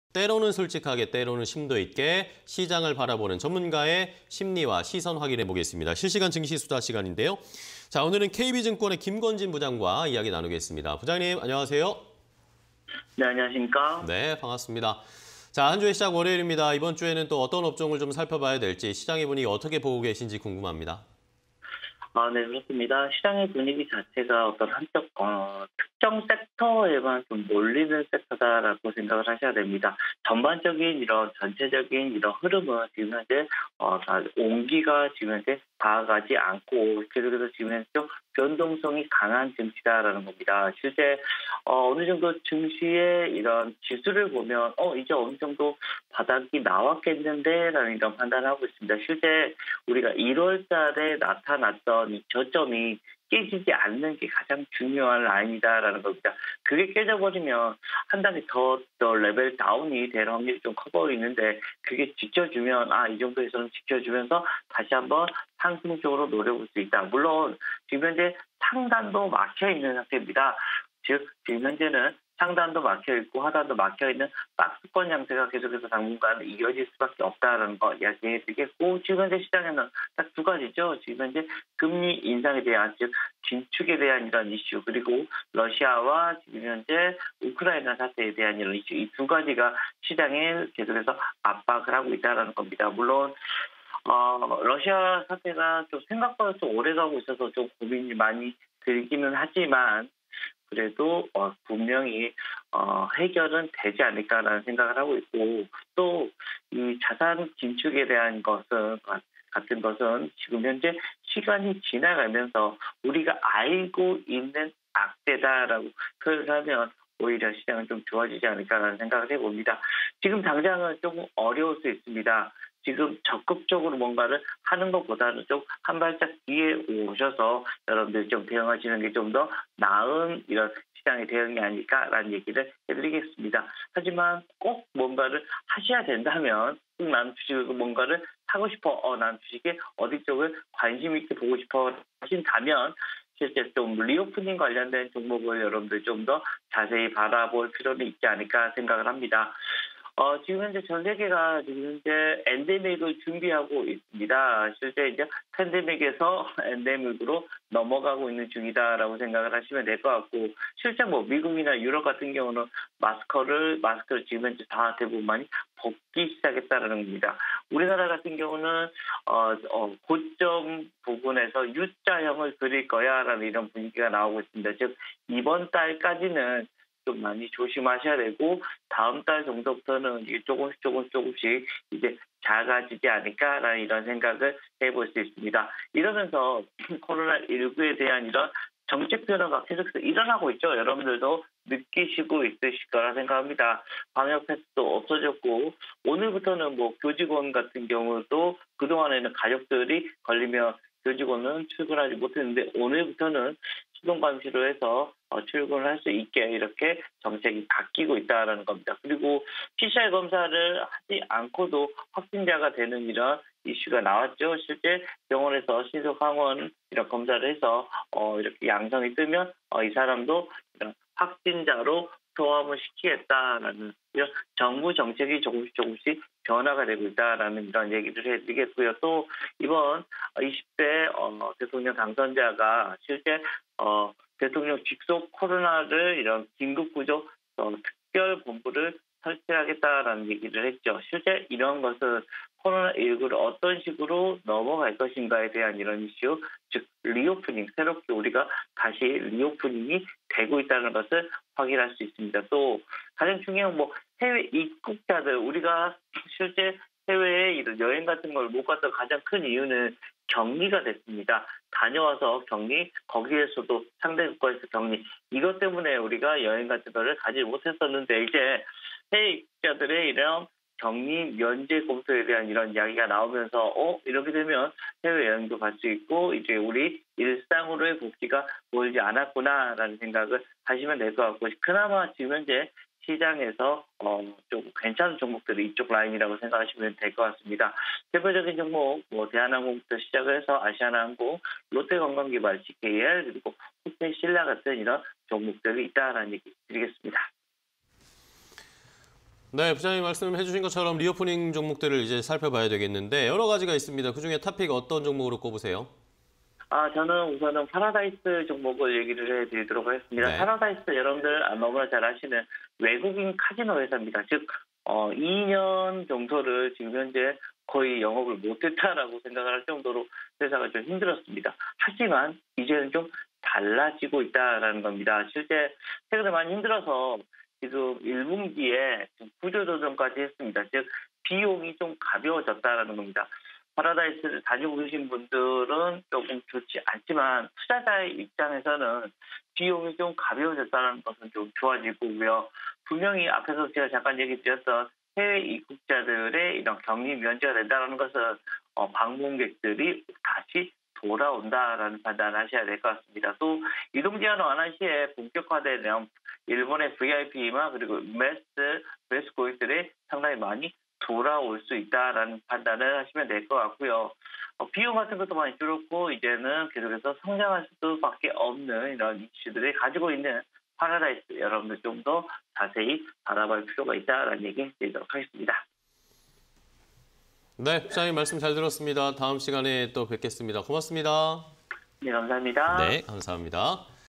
파라다이스(034320)... 해외 입국자 격리 면제… '리오프닝주' 주목_실시간 증시 수다 (20220314)
Q. 봉쇄 풀린 국경… ' 리오프닝주' 빛 보나?
- 코로나19 관련 정책기조 변화 예상
- 대통령 직속 '코로나19 긴급 구조 특별본부' 설치
- '백신 접종' 해외 입국자 7일 격리 면제
- 대한항공·아시아나항공·롯데관광개발·GKL·호텔신라 등

파라다이스(034320)
- 1Q 구조조정으로 가벼워지는 인건비
- 2Q 수요 증가 본격화 예상… 실적 개선 기대
- 리오프닝 기대감에 수혜
- 비카지노 매출 회복 예상… 평균객실단가 점진적 개선